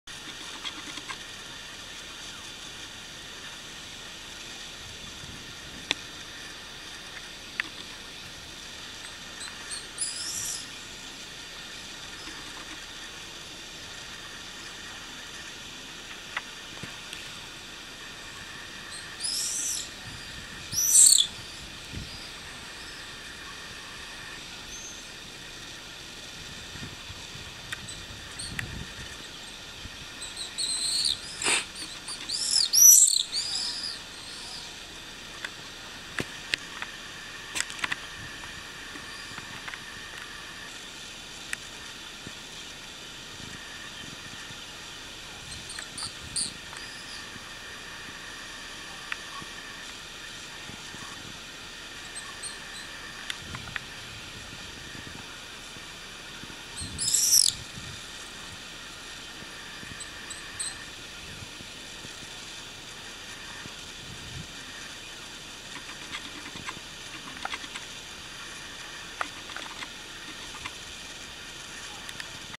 rondini
nel video le rondini nel centro storico di Civitaluparella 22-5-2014